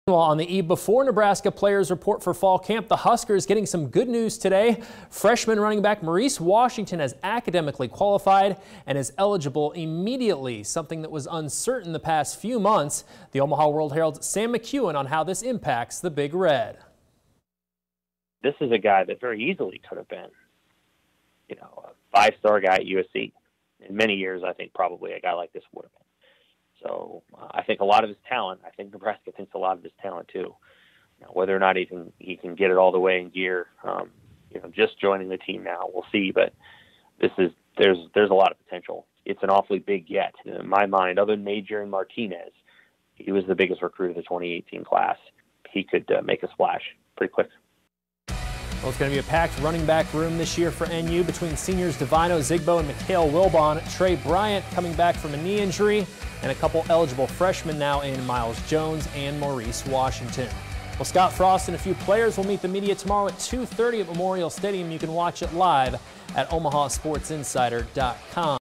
OWH's Sam McKewon on Maurice Washington's Immediate Eligibility
The Omaha World-Herald's Sam McKewon gives his thoughts on Nebraska running back Maurice Washington being declared immediately eligible.